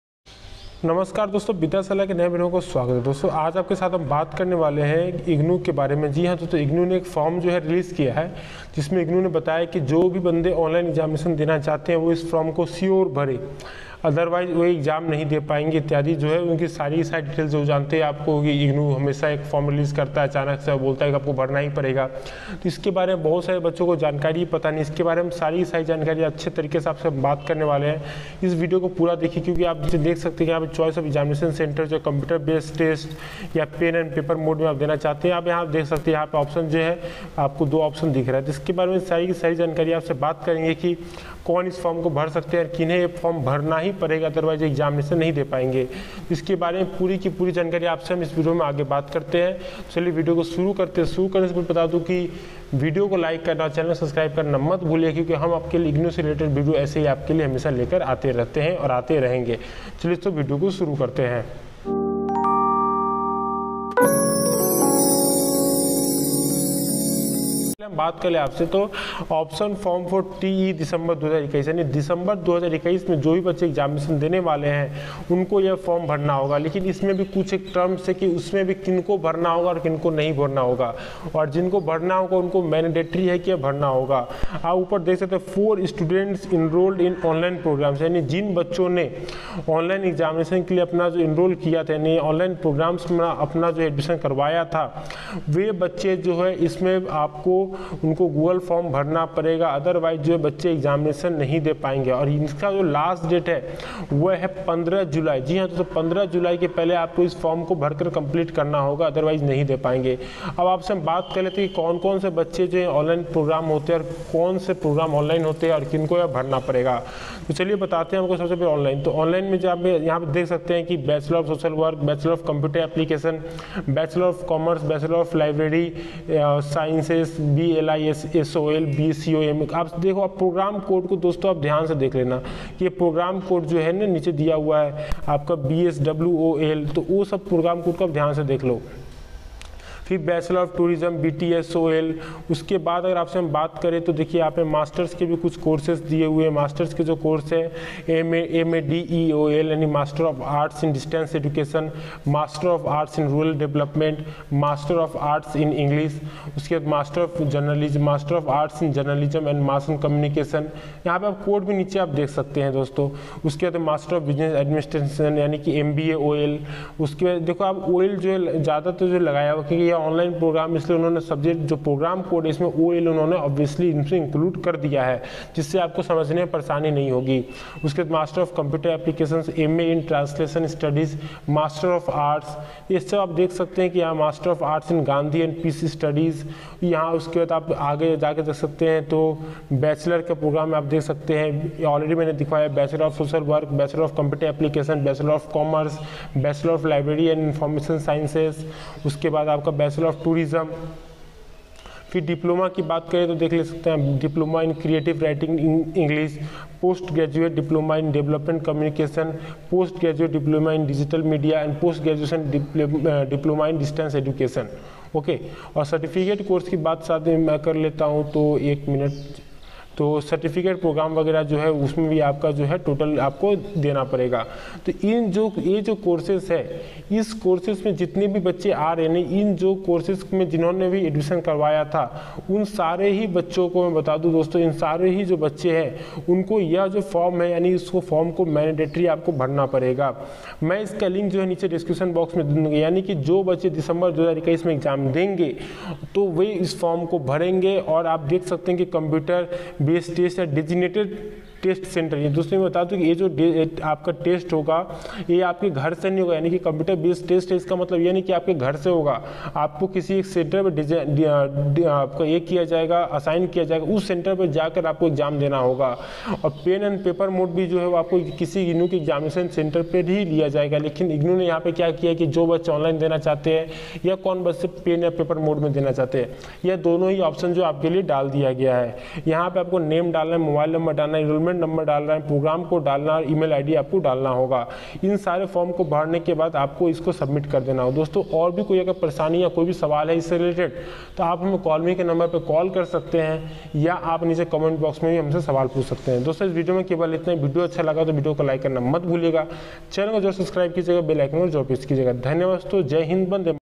Breaking News : IGNOU 2022 Online Exam देने के लिए करें जल्द आवेदन | Submit कीजिए यह फॉर्म
Namaskar Dosto, Iss Video mein main aapse baat kr rha hu, IGNOU ke online Enrolled students ke baare mein, jin bacchon ne online program mein enrolled kiya tha, unhe yeh form bhrna mandatory hai , otherwise we dec 2021 ka exam nahi de payenge

Call Me App Link for Any Query :-
vidyashala@cm4